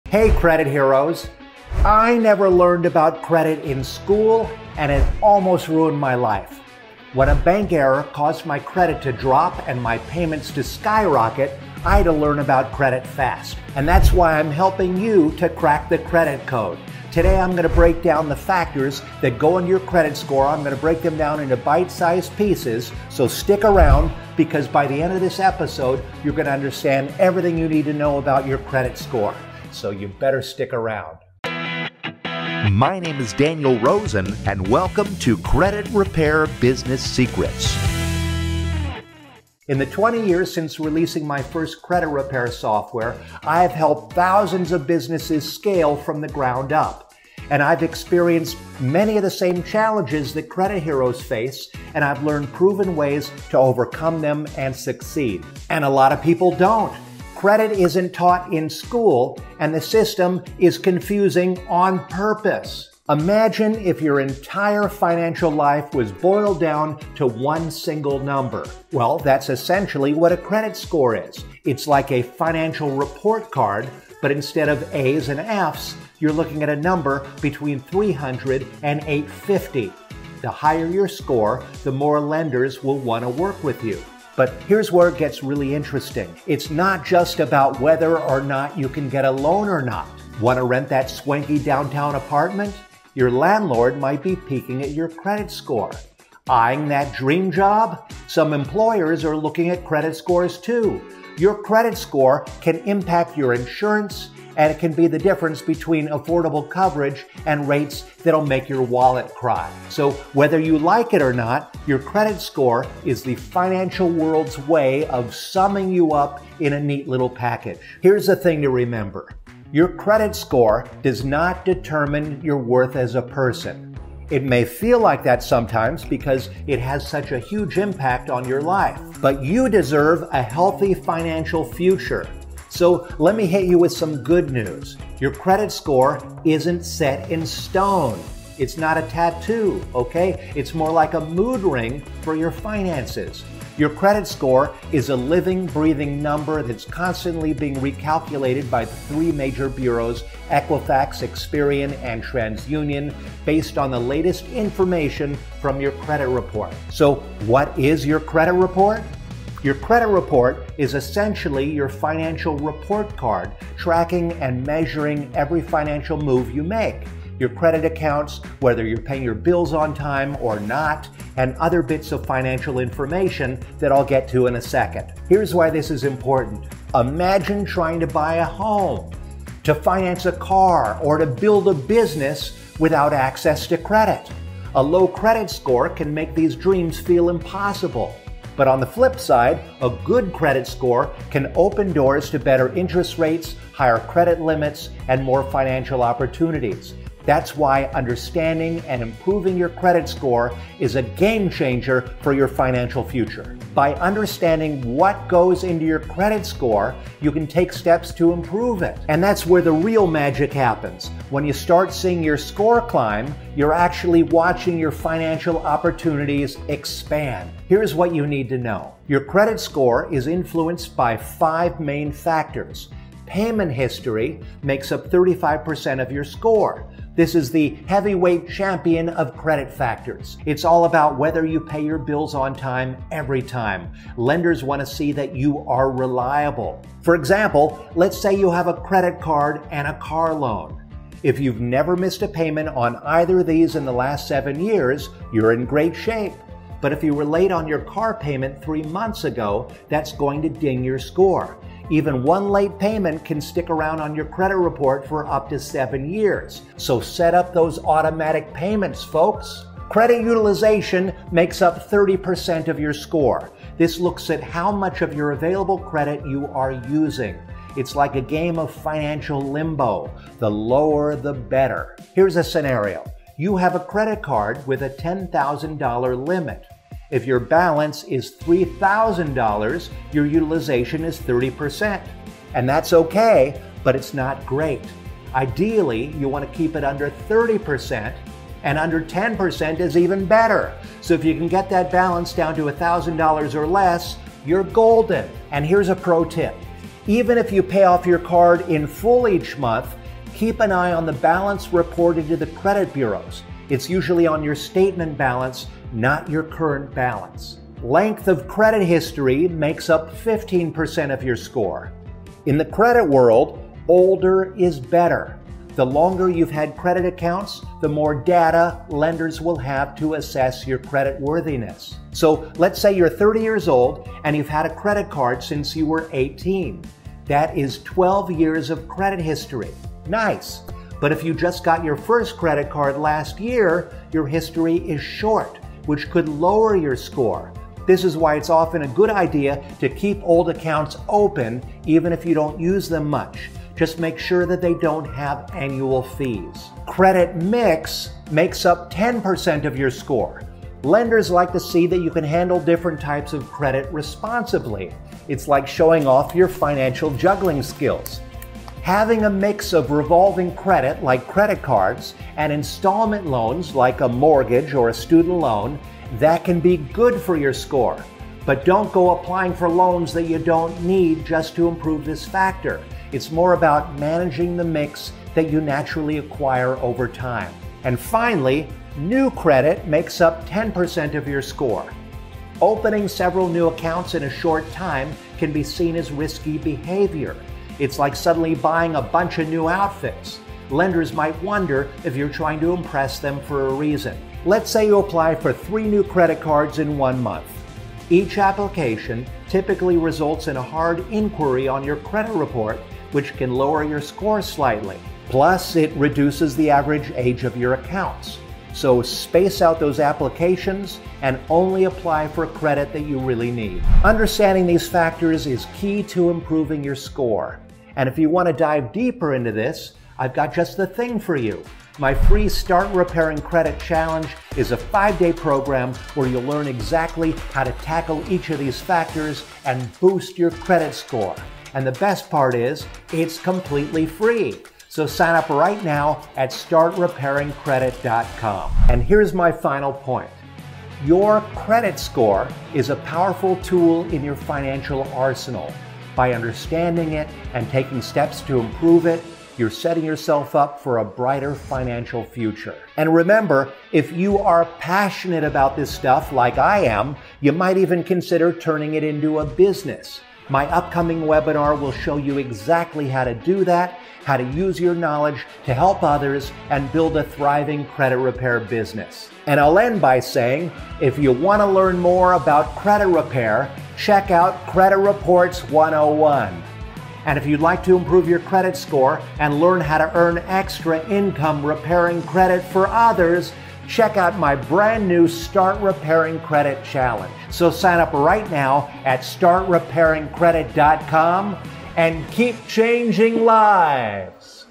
Credit Score 101: Everything You Need to Know
I never learned about credit in school, and it almost ruined my life.

So today, I'm sharing everything you need to know about credit scores: what they are, the 5 key factors that determine them, and how you can raise them. By the end of this episode, you'll understand everything you need to know about your credit score, so you never end up like me!

Let’s jump right in.

Key Takeaways:

00:00 Intro
00:39 What Is a Credit Score?
02:23 What Is a Credit Report?
03:37 5 Key Factors That Determine Your Credit Score
07:18 My Final Point
07:49 Outro